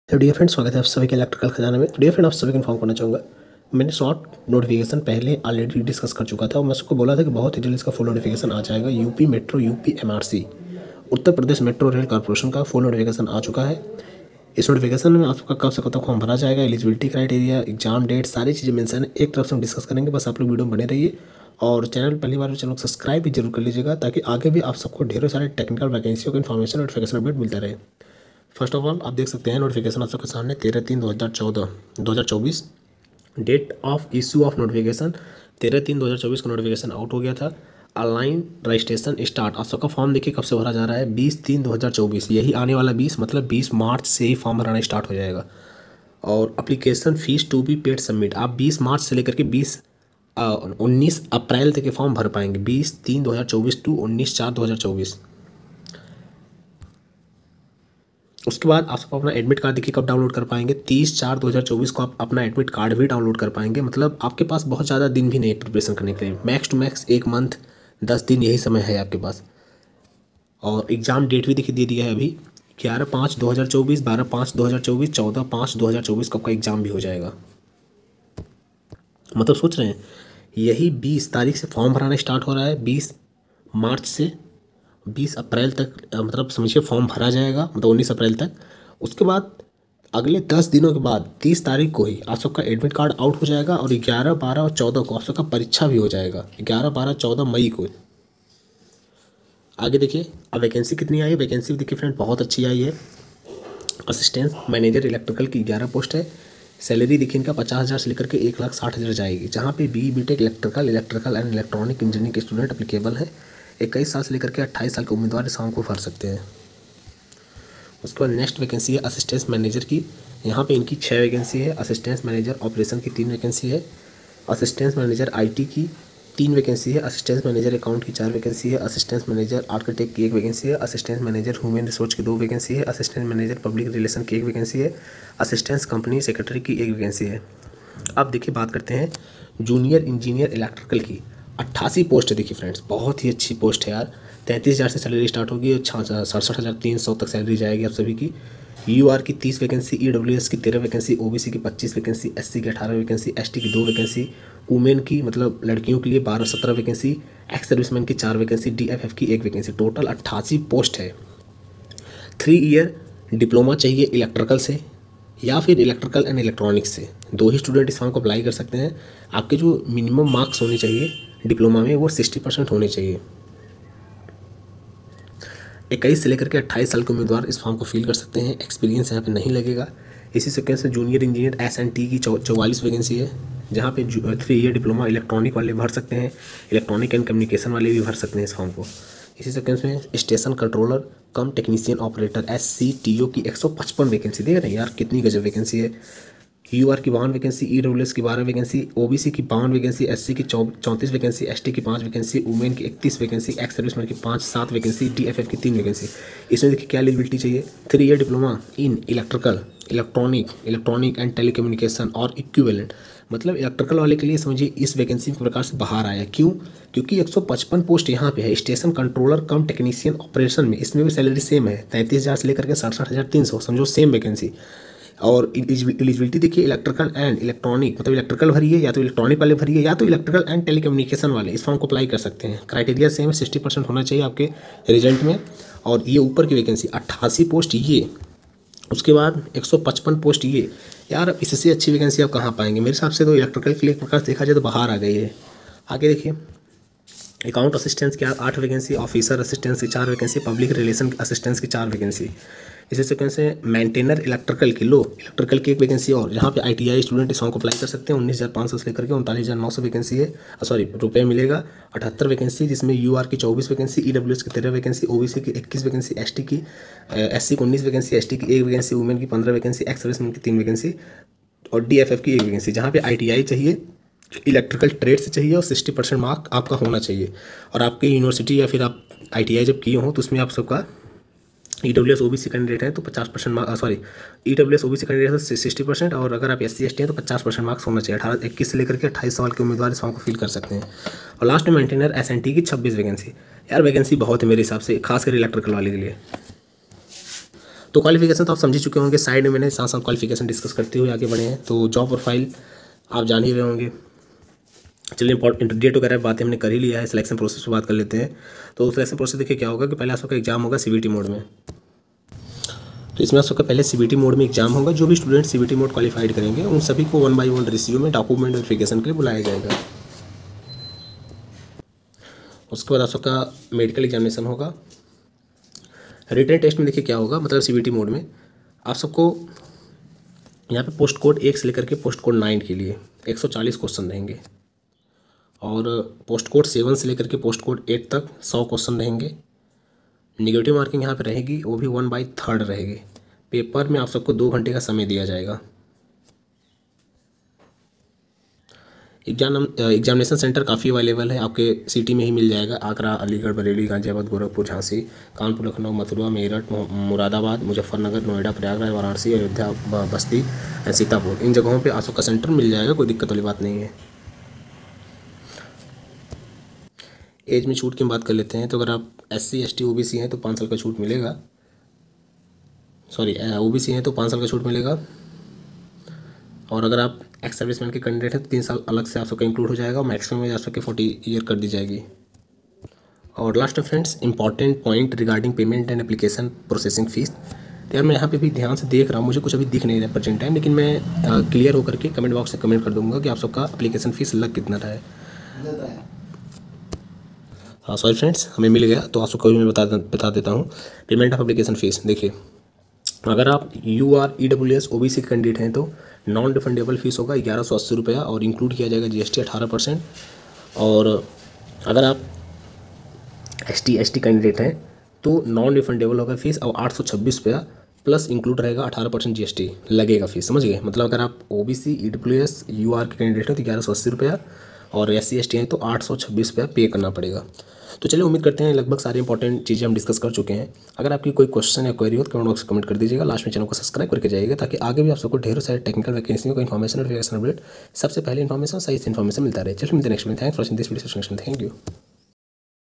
upmrc 2024 full notification out, upmrc vacancy details, total Post -439 for diploma BTech iti
upmrc vacancy details, selection process, sallabus exam date, exam pattern, eligibilty

UPMRC RECRUITMENT FULL NOTIFICATION OUT

DIPLOMA / BTECH IN RELEATED STREAMS

 Assistant Manager, Junior Engineer, SCTO, Maintainer

 EXAM DATE - 11,12 & 14 May 2024

LAST DATE - 19 April 2024